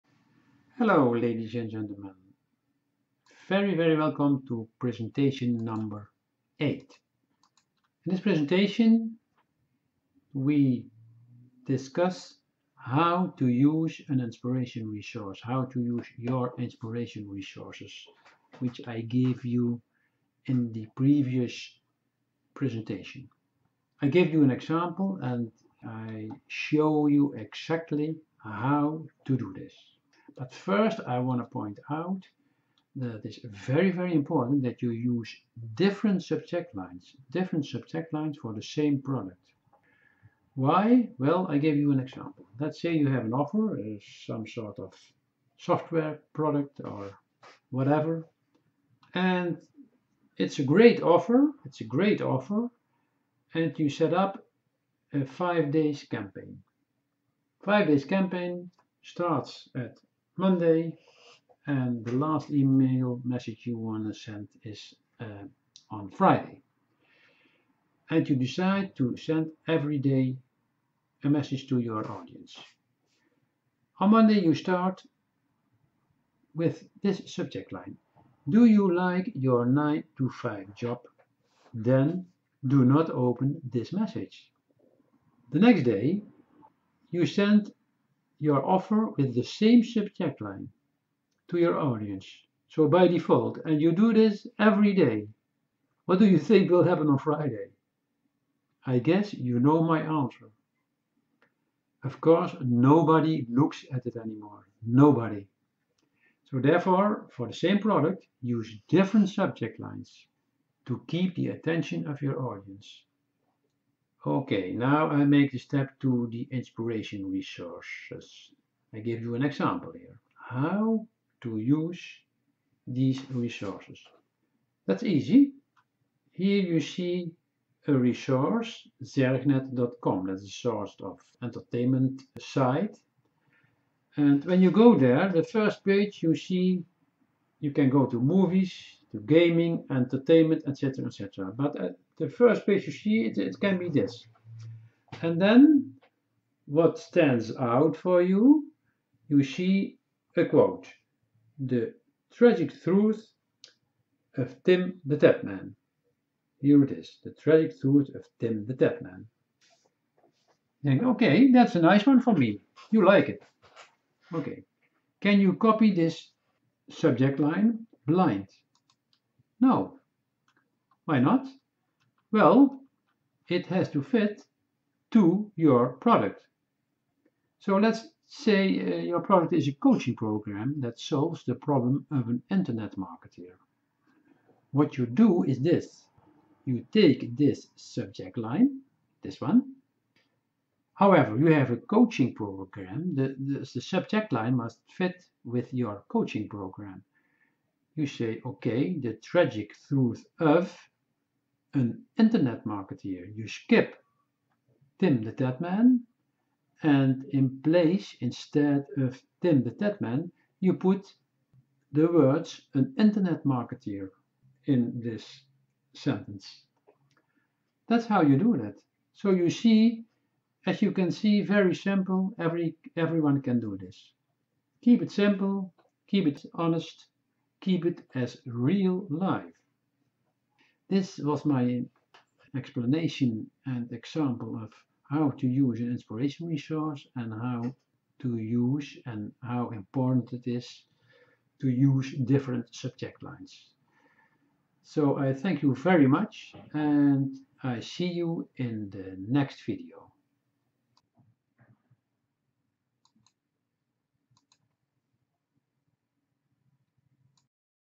E-Mail Power Machine - How To Use Email Marketing Inspirational Resources (V #08)
How to use inspirational resources to keep the attention of your audience.